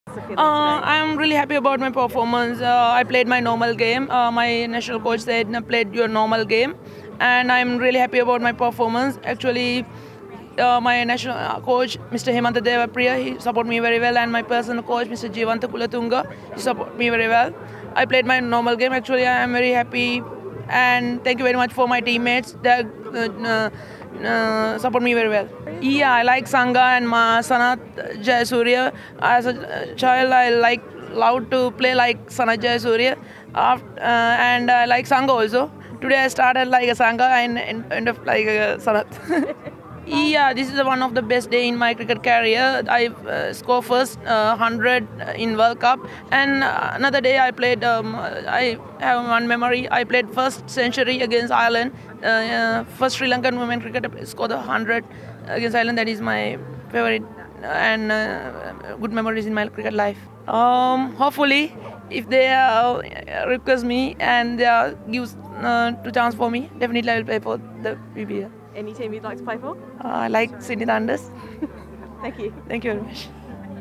Proud Athapaththu reflects on her all-time knock
After her record-breaking century against Australia, Chamari Athapaththu reveals hero her Sri Lankan batting heroes and admits she'd jump at a stint in the WBBL